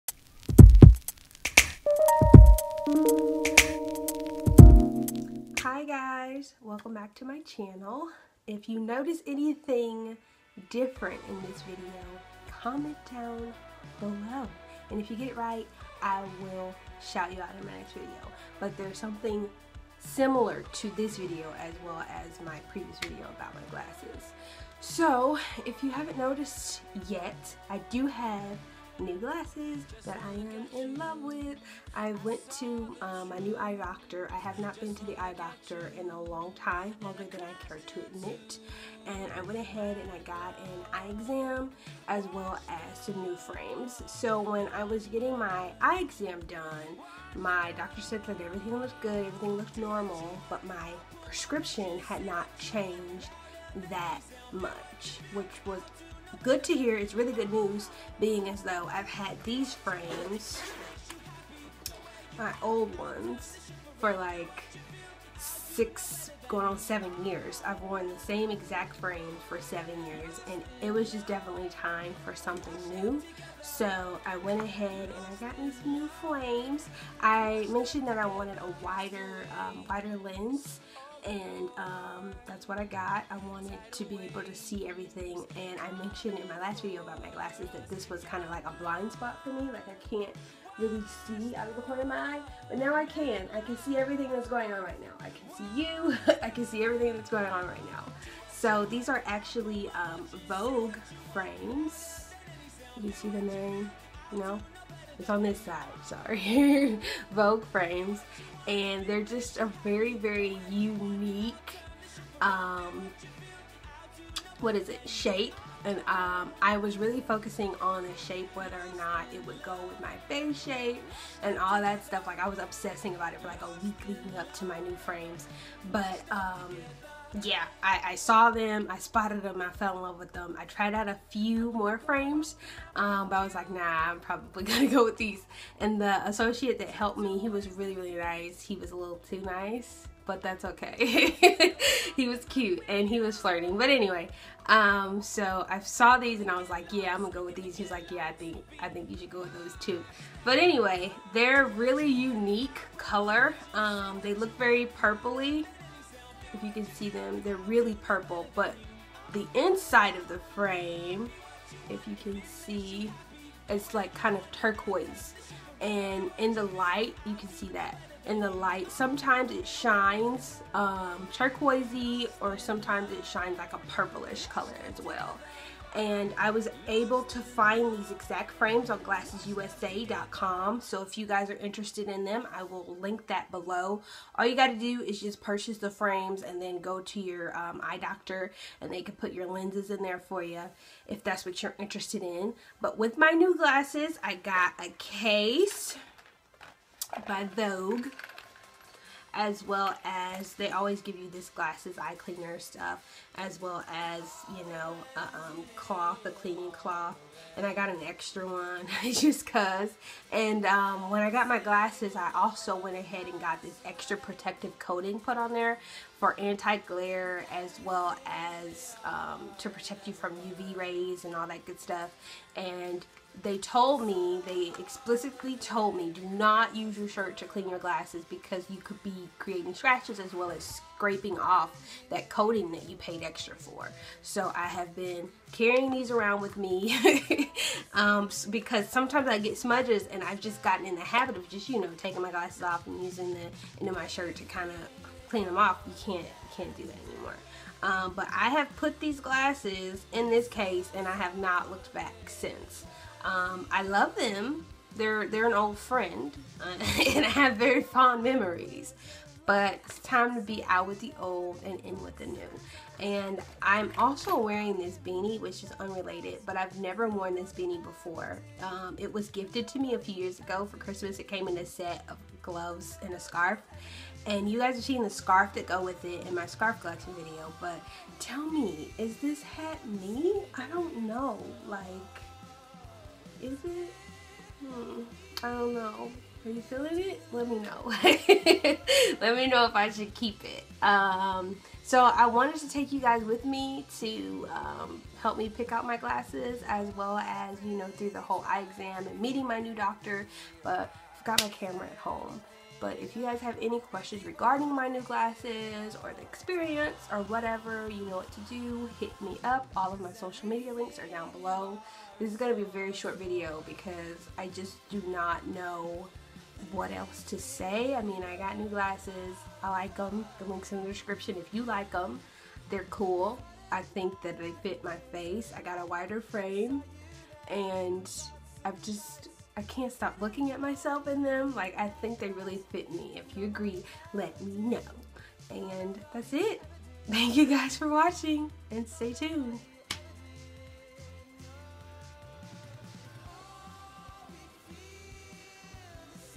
MY NEW GLASSES/FRAMES
Hey guys! I finally went to the eye doctor and got some new frames with a new prescription. I'm in love with them, and it feels so good to have something new 😊🤓